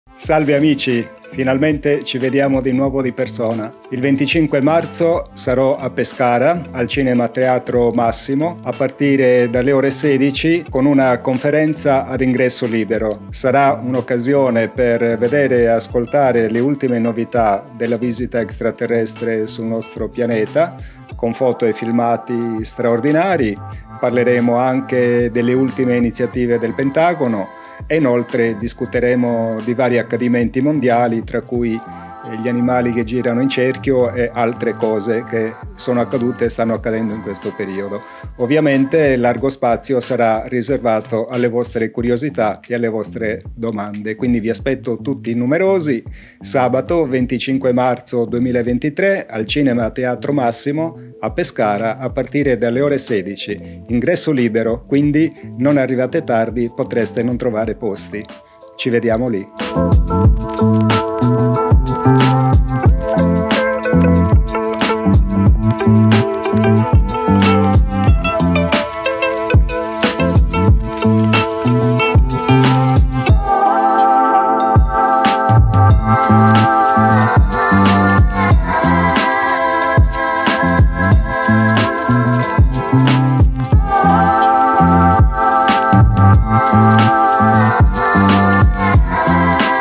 Trailer - Pier Giorgio Caria a PESCARA sabato 25 MARZO 2023 alle ore 16,00 [INGRESSO LIBERO]
🎦Tipologia: Lancio pubblicitario 〰️ 📅Data: 25/03/2023
⭐️Titolo: ATTIVITÀ EXTRATERRESTRE ED EVENTI MONDIALI
► 👍Metti mi piace ai miei video e condividili sui Social!
📌Luogo: Cineteatro Massimo
Via Caduti del Forte, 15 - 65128 Pescara

Il rincorrersi degli eventi aumenta sempre di più la sua velocità. La guerra in Europa, lo scontro geopolitico ed economico tra le grandi potenze, si intensificano enormemente determinando una situazione rischiosissima che potrebbe scatenare una terribile terza guerra mondiale con il rischio non molto remoto, dell’uso dell’arma atomica.

A questo si aggiungono disastri climatici e geologici culminati con il terribile terremoto che ha causato oltre 50.000 cinquantamila vittime tra Turchia e Siria. Sullo sfondo di questa drammatica situazione la visita extraterrestre è un protagonista occulto ma non troppo ed eccezionali avvistamenti di astronavi si moltiplicano giorno dopo giorno.

Ma il potere non sta certo a guardare, e quando parliamo di potere non possiamo che riferirci agli Stati Uniti d’America. La politica della rivelazione pilotata da parte del Pentagono prosegue a tappe forzate con altri rilasci di materiale ufologico fotografico e filmato di origine militare e con la creazione di istituti farsa che stanno conducendo verso la realizzazione del famigerato progetto Blue Beam.

Le immagini di animali che girano in cerchio sino a morire per lo sfinimento hanno fatto il giro del mondo, cosa vuole annunciarci la natura? Verso dove va l’umanità? Cosa hanno in mente di fare gli extraterrestri? Come porci di fronte a questi drammatici scenari? Di questo e di altro parleremo in questo nuovo incontro a ingresso libero che terrò a Pescara sabato 25 marzo 2023.

Non mancherà la proiezione di immagini di straordinari avvistamenti e un ampio spazio alle vostre domande. Vi aspetto!

📣Rimani aggiornato su:

🎬Crediti:

👨‍💻Relatore: Pier Giorgio Caria
💻Regia: Thomas Matteucci
💻Assistente alla regia: Fabio Maggiore
🎥Cameramen: Thomas Matteucci, Fabio Maggiore
🖥Post Produzione e Editing: Thomas Matteucci
🖥️ Motion graphic: Giorgio Di Stefano
📸 Fotografia: Jacopo Bonfili

©️ Pier Giorgio Caria